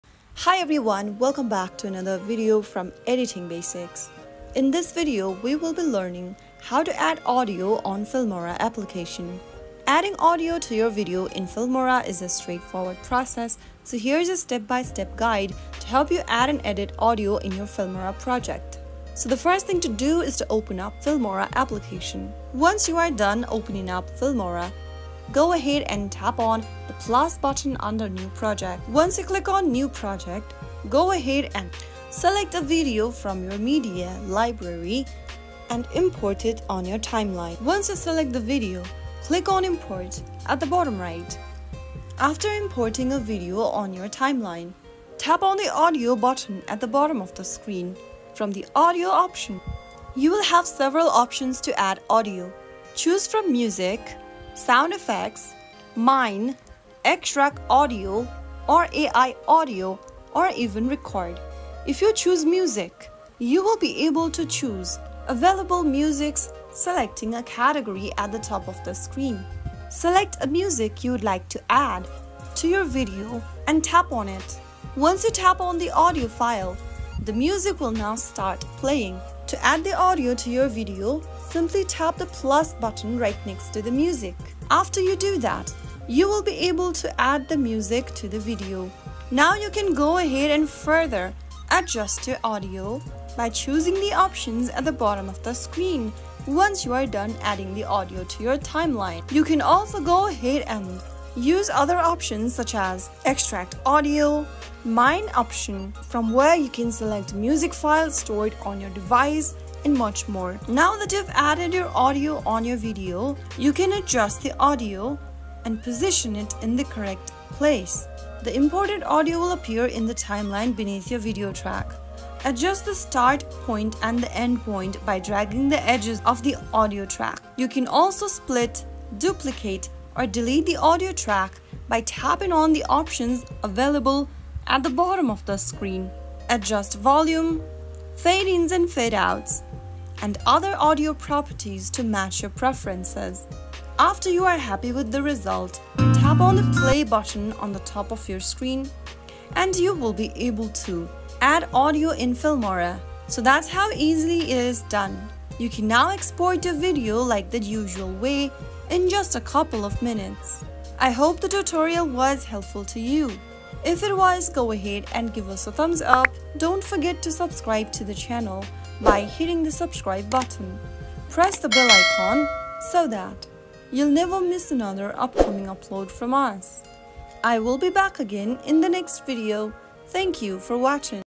How to Add Audio in Filmora App 2024?
Welcome to our tutorial on how to add audio in the Filmora app! Enhancing your videos with music, voiceovers, or sound effects can make them more engaging and professional.

Join us as we guide you through the process of adding audio in Filmora, helping you create captivating and high-quality videos!